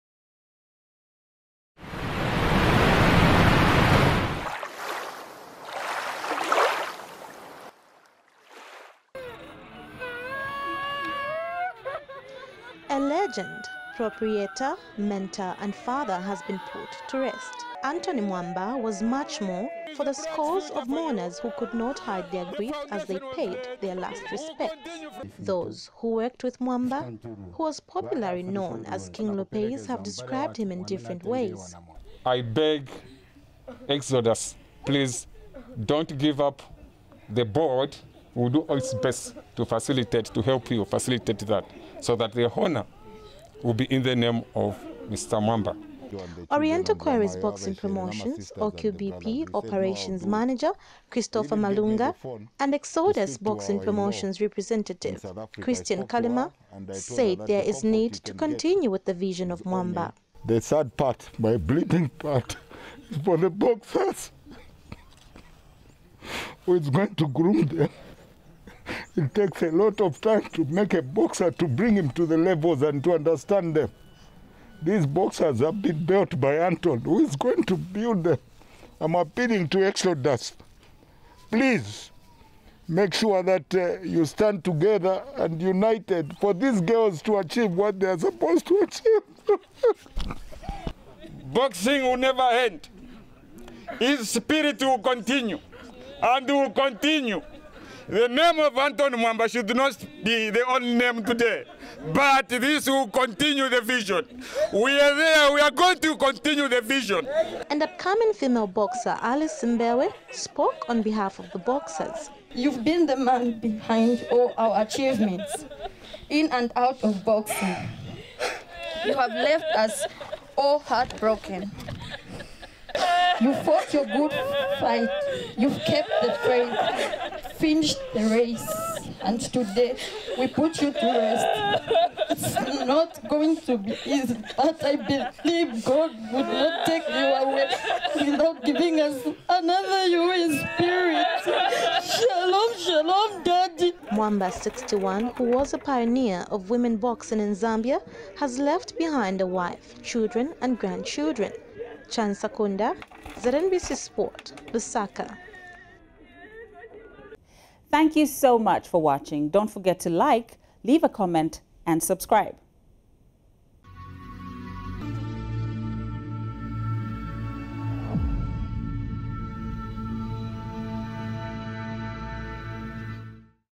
BOXING ANTHONY MWAMBA PUT TO REST
Veteran Boxer and Exodus Boxing Promotions proprietor ANTHONY MWAMBA has been put to rest.

MWAMBA who succumbed to COVID-19 on Thursday the 21st of January 2021 was buried at the Memorial Park in Lusaka.

Scores of mourners from all sectors of society and the boxing fraternity paid their last respects to MWAMBA who was the pioneer of women boxing in Zambia.

And the Zambia Professional Boxing and Wrestling Control Board -ZPBWCB- Chairperson, Brigadier General ABYNOT MULELA said the board will do everything in its power to facilitate for the continuous operation of Exodus promotions..

Oriental Quarries Boxing Promotions -Manager CHRISTOPHER MALUNGA said the death of  Mwamba is a blow to the boxing fraternity.

MALUNGA has since appealed to Exodus to ensure that they continue with MWAMBA's vision of grooming young male and female boxers in the country.

Exodus Boxing Promotions Representative CHRISTIAN KALIMA said MWAMBA's passion and vision for Boxing in Zambia will be carried on.